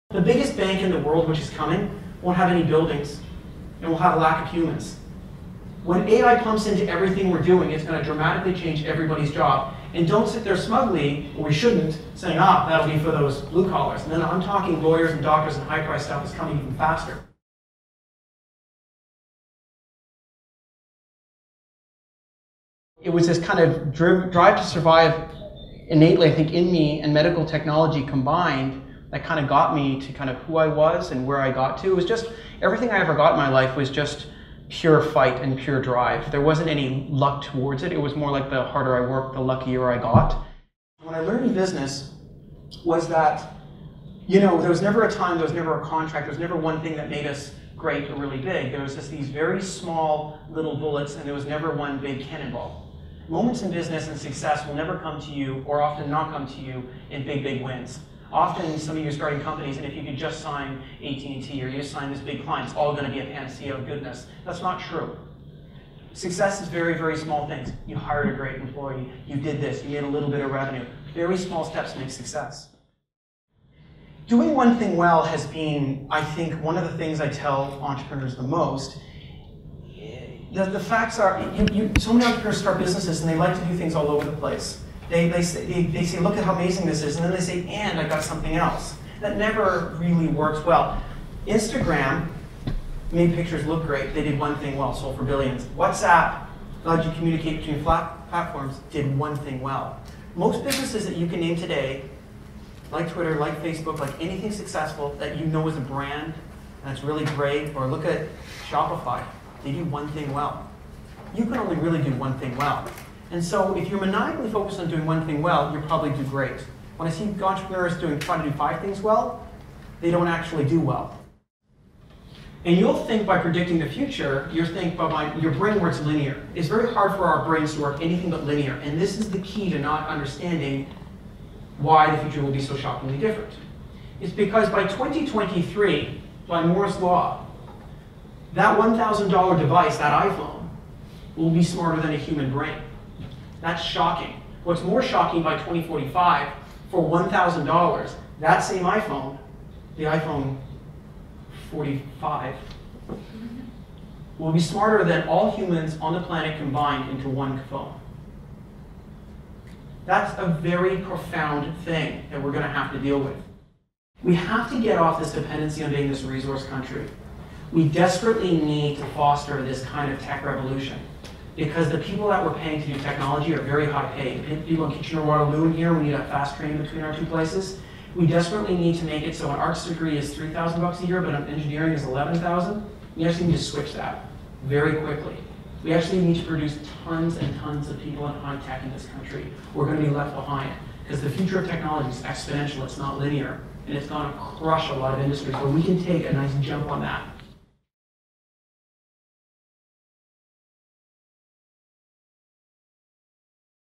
Highlights from Michael Hyatt talk to Ivey Alumni - Toronto Chapter
Michael Hyatt talks to Ivey Alumni - Toronto Chapter event. He shares his view on opportunity.